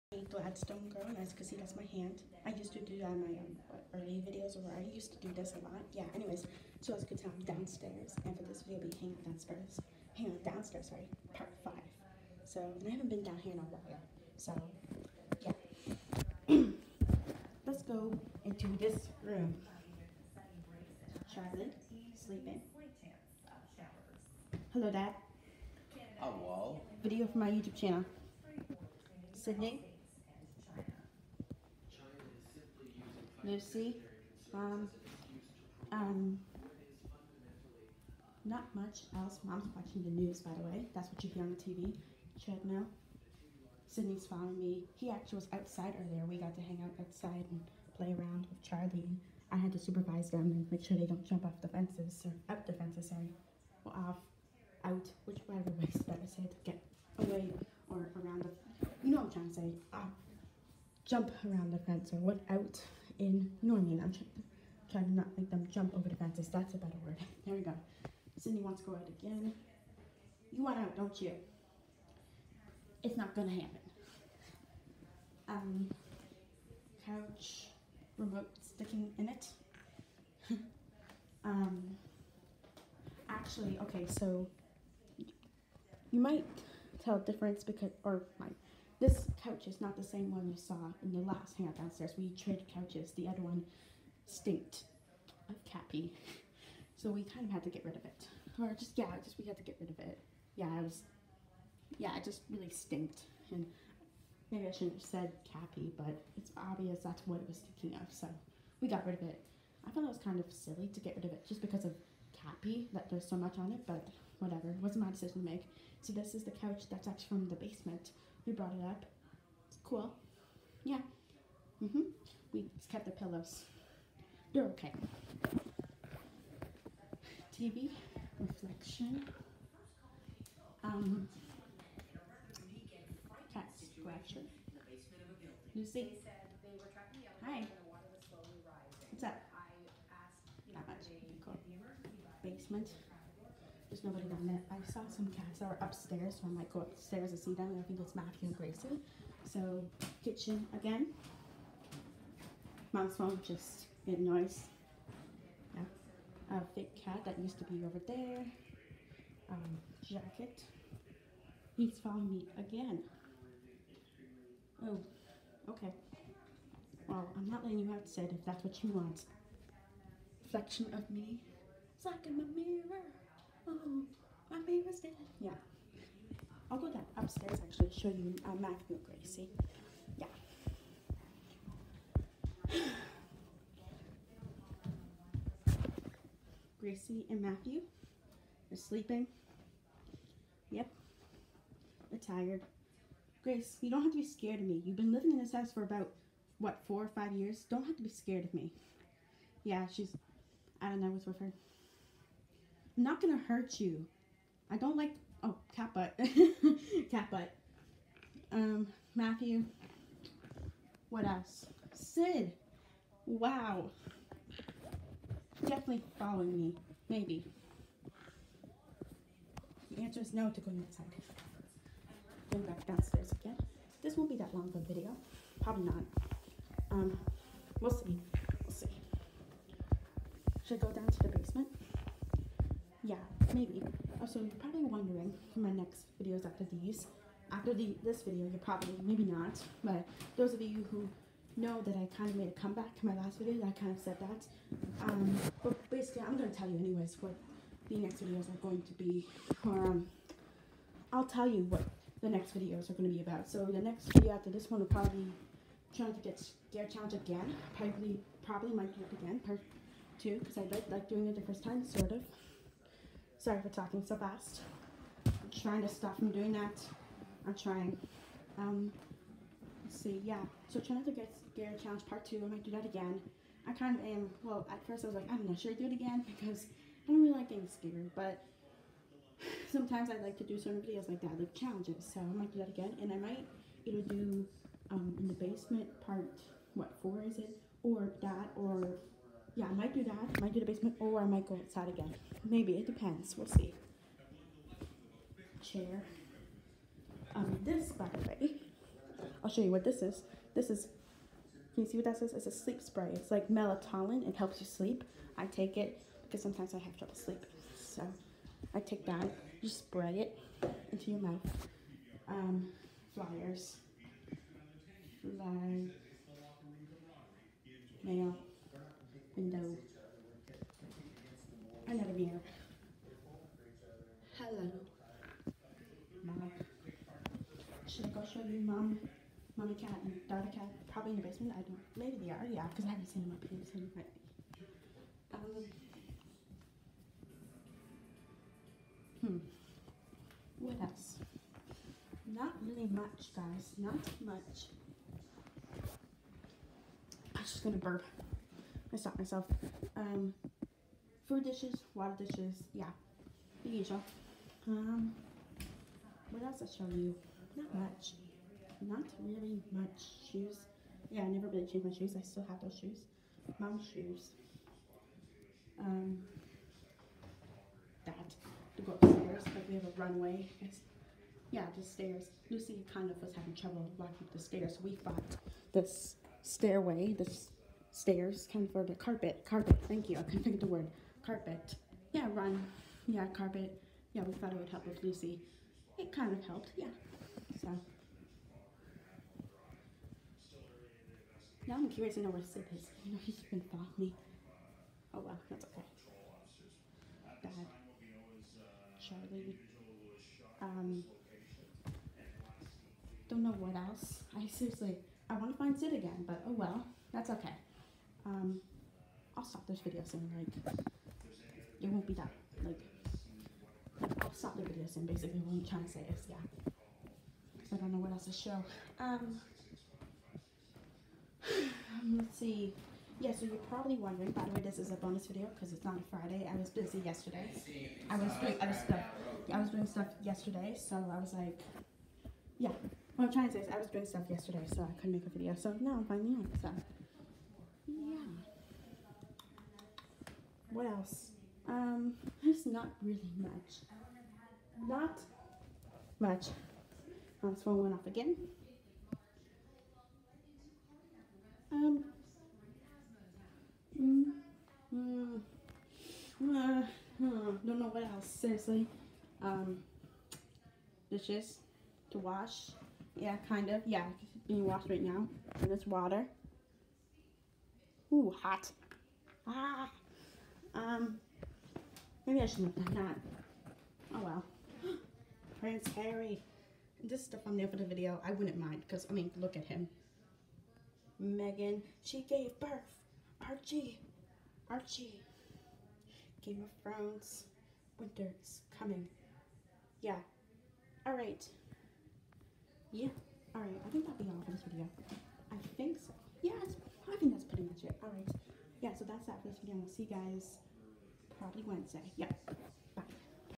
Hanging out downstairs part 5
Decided to do a part 5 to downstairs videos because it’s fun to do and I like doing them. The pets in this video are Sidney Charlie Matt grace and Lucy mom and dad were there too.